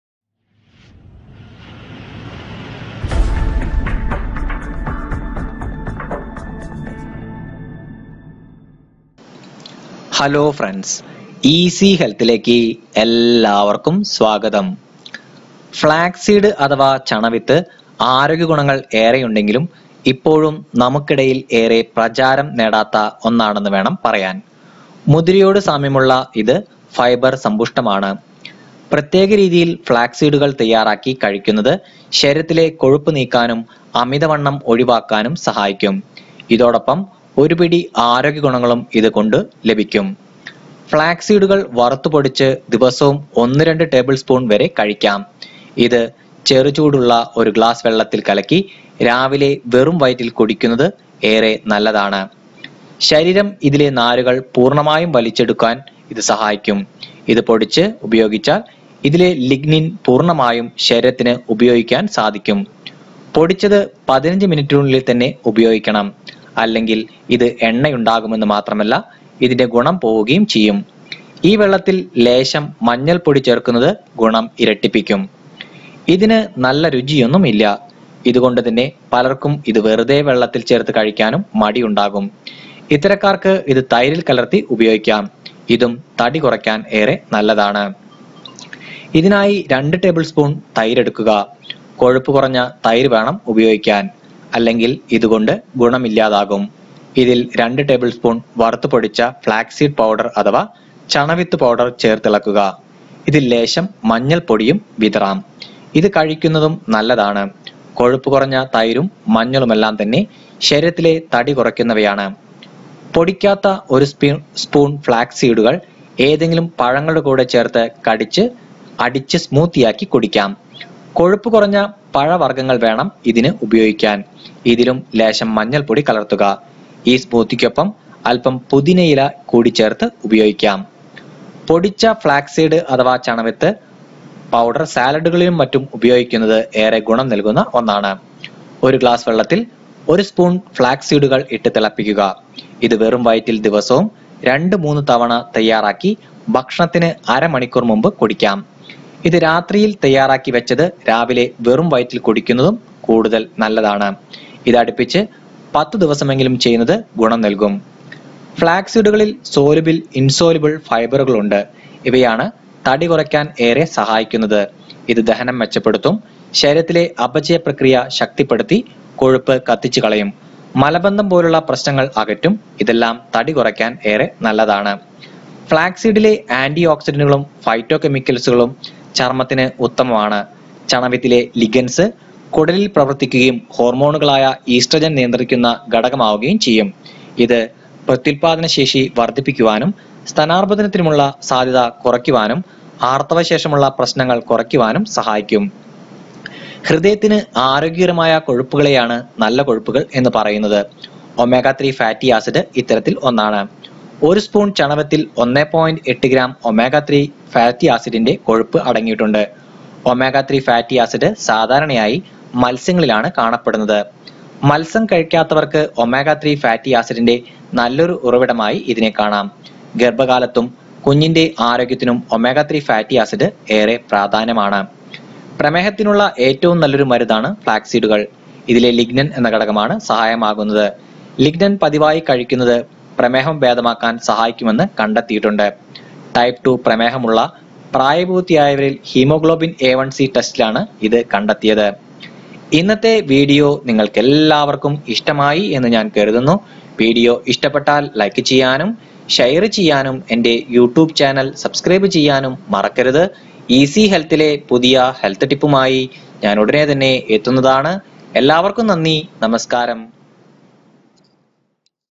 ശരീരത്തിലെ കൊഴുപ്പു നീക്കാനും അമിത വണ്ണം ഒഴിവാക്കാനും||Health Tips Malayalam
പ്രത്യേക രീതിയില്‍ ഫ്‌ളാക്‌സ് സീഡുകള്‍ തയ്യാറാക്കി കഴിയ്ക്കുന്നത് ശരീരത്തിലെ കൊഴുപ്പു നീക്കാനും അമിത വണ്ണം ഒഴിവാക്കാനും സഹായിക്കും. ഇതോടൊപ്പം ഒരു പിടി ആരോഗ്യ ഗുണങ്ങളും ഇതു കൊണ്ടു ലഭിയ്ക്കും. ഫ്‌ളാക്‌സ് സീഡുകളില്‍ ആല്‍ഫ ലിനോയിക് ആസിഡ് അടങ്ങിയിട്ടുണ്ട്. . ആസ്ത്മ, പ്രമേഹം, ഹൃദയരോഗങ്ങള്‍, ആര്‍ത്രൈറ്റിസ് എന്നിവ തടയാനും കുടലിലെ ക്യാന്‍സര്‍ തടയാനും ഇത് സഹായിക്കും.How To Use Flax Seeds To Reduce Belly Fat And Weight